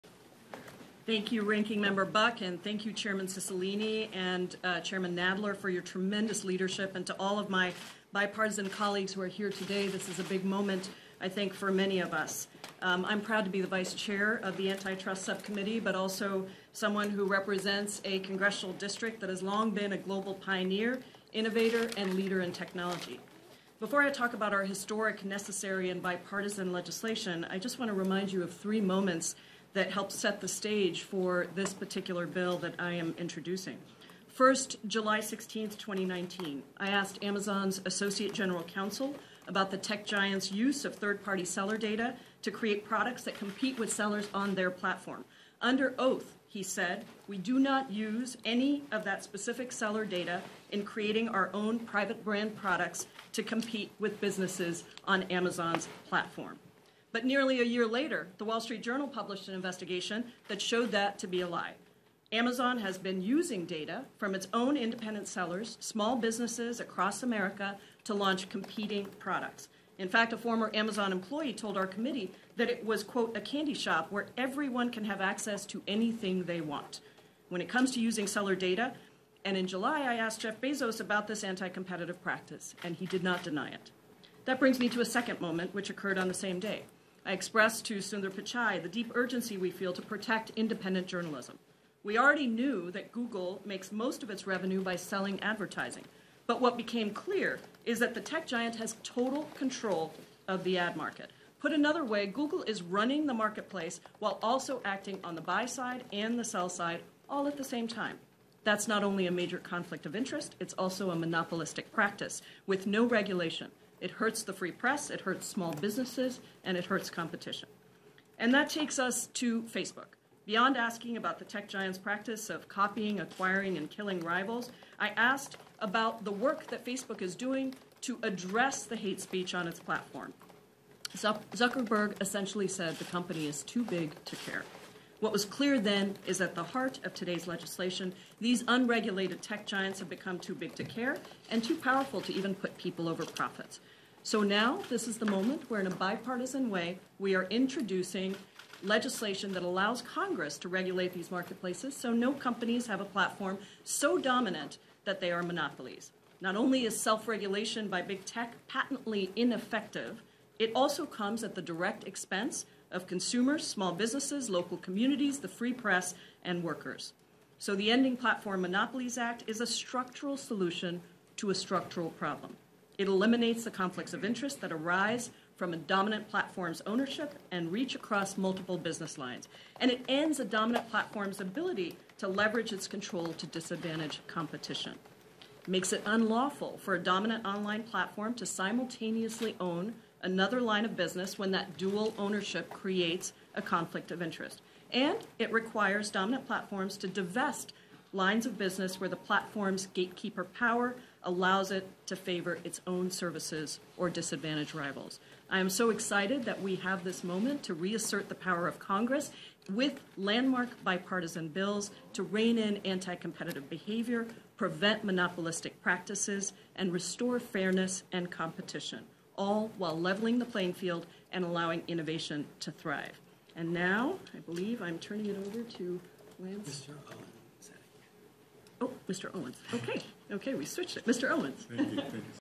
Democrat Rep. SLAMS Big Tech CEOs: "They're too big to care and put profits over people"
Pramila Jayapal (D-WA) spoke during a presser last week led by members of the House Judiciary Committee in an effort to promote bipartisan legislation that holds unregulated Big Tech monopolies accountable for anti-competitive conduct.